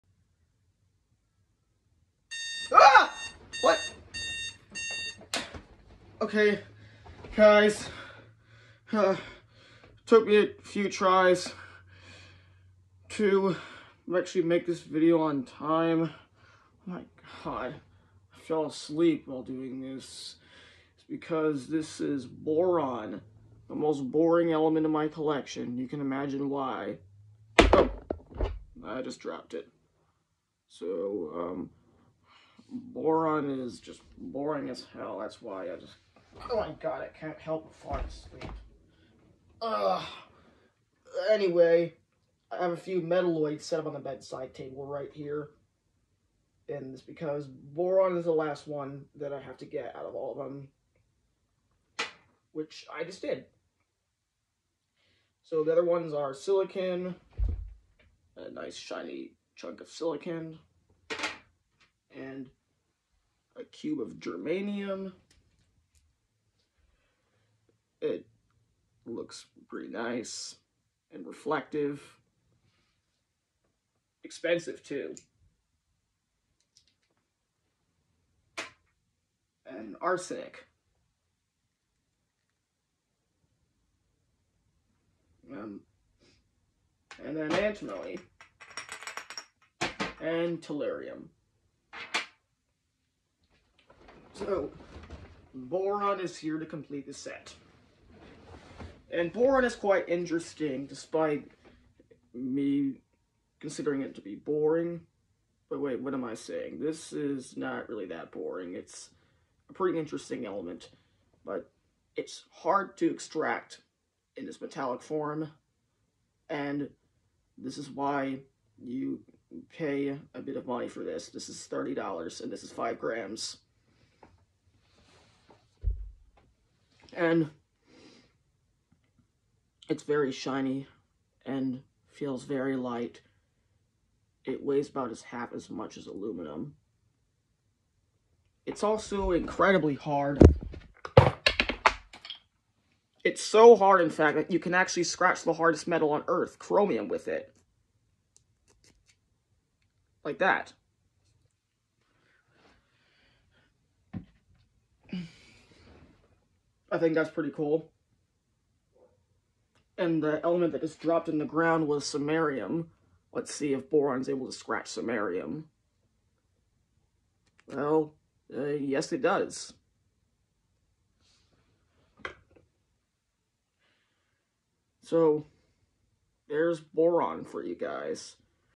My Element Collection Episode 5: Boron (The Most Un-boring Element) (Reupload)
Boron is an element that is a metalloid, meaning it combines properties of metals and nonmetals. It may seem boring, hence its name, but it is quite interesting. Unlike most elements, most boron on Earth, probably including my sample, was formed when high-energy cosmic rays collide with carbon or oxygen atoms, or other heavier elements. It is the second hardest element at 9.2 on the Mohs Scale. Carbon in the form of diamond comes in at first place at 10 on the scale. Boron is also quite difficult to extract, so it suffers from that and its lack of demand from industries, which is why boron costs $3500 per kilogram. My sample was close to 5 grams and costed $30. It is also incredibly difficult to work with. Making items out of boron is difficult, so a centimeter cube of it costs about $150, even though it is less than $10 worth of the element itself. It is a very light metalloid, so my sample also feels like nothing